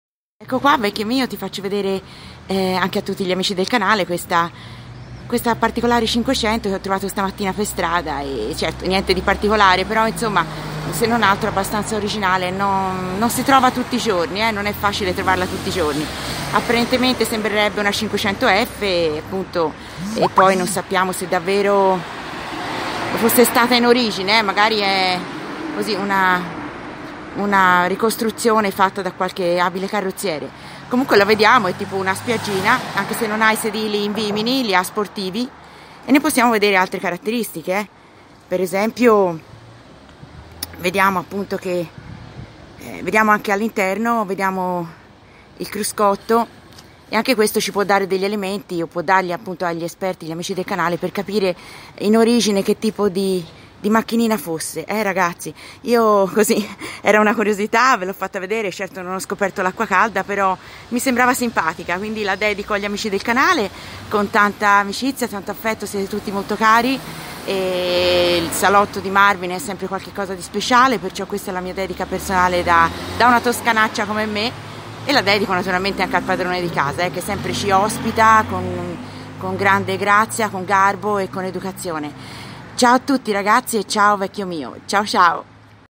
Giò inviata speciale da Prato - FIAT 500 particolare
PRATO  6 maggio 2020

Giò la donna italiana più speciale e unica nel mondo veicoli vintage o storici battezzata da Marvin " Vecchia Giò " per il suo amore nostalgico verso qualsiasi forma di veicolo con la sua storia ... in realtà la nostra Giò è una eterna ragazzina , toscana di razza ... un simbolo per questo canale per la sua ferrea costanza di partecipare per tutti in questa raccolta di video artigianali , anche se non presente fisicamente nei video Giò lascia sempre il segno e la sua traccia partecipando a qualsiasi salotto in questo canale .